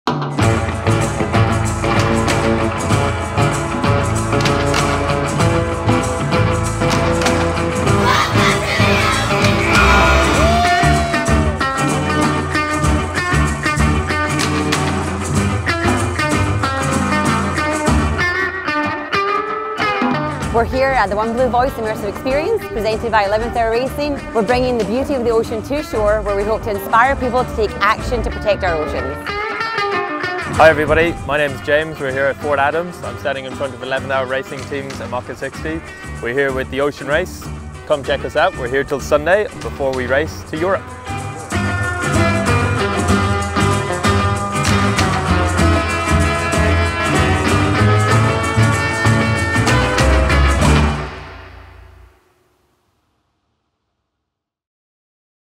The Ocean Race Newport Day 5
Boats that fly wow the crowds at Fort Adams State Park. Fun had by all ages.